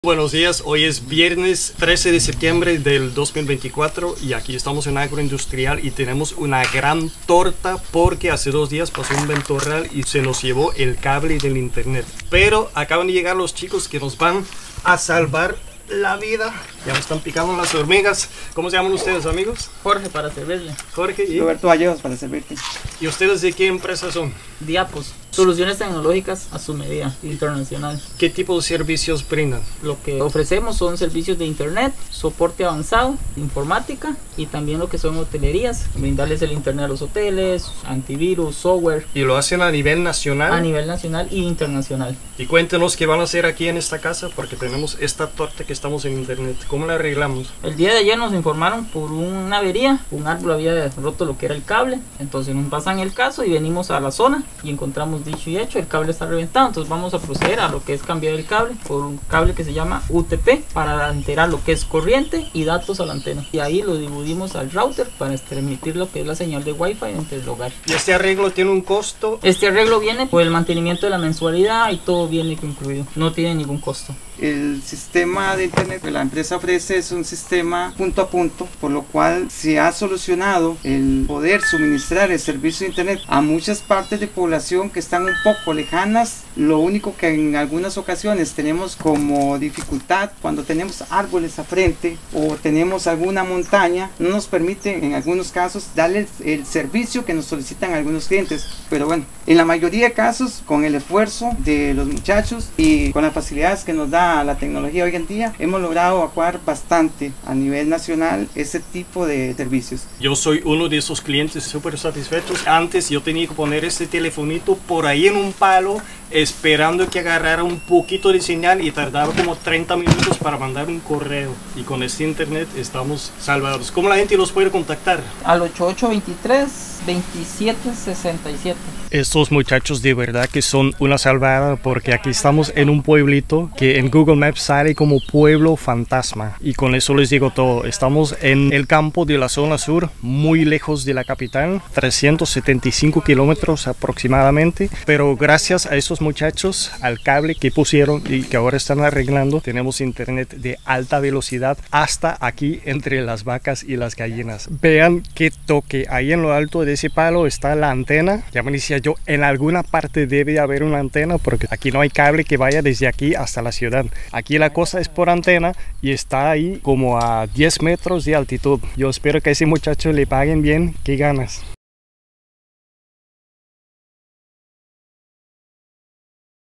Estos chicos traen INTERNET al campo / These folks bring INTERNET to the countryside
VIDEO ACTUALIZADO. En una versión anterior de este video, se mostraba un número de teléfono equivocado. El número correcto de D-APOS es (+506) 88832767. Llámenlos si viven lejos de San José y necesitan internet en su casa o empresa. El servicio es excelente y el costo es cómodo.

UPDATED VIDEO. A previous version of this video showed the wrong phone number. The right number of D-APOS is (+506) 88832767. Call them if you live far from San José and need internet at home or in your business. Their service is outstanding and the fees are affordable.